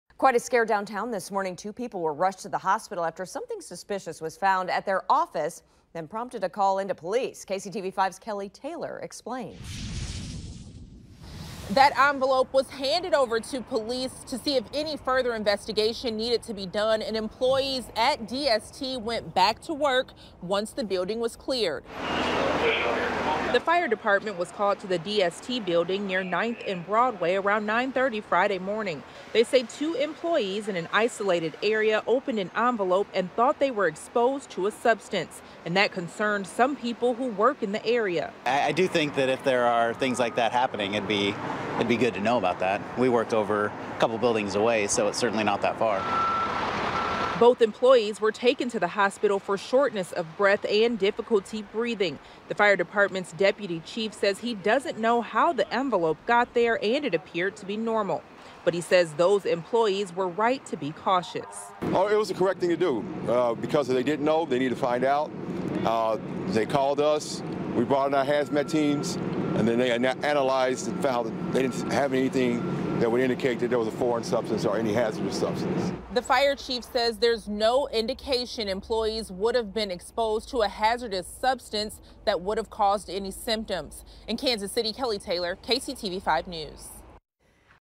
DST employees report sickness after opening envelope with white powder
No hazardous materials were found after several employees reported breathing issues inside a downtown office building, firefighters said.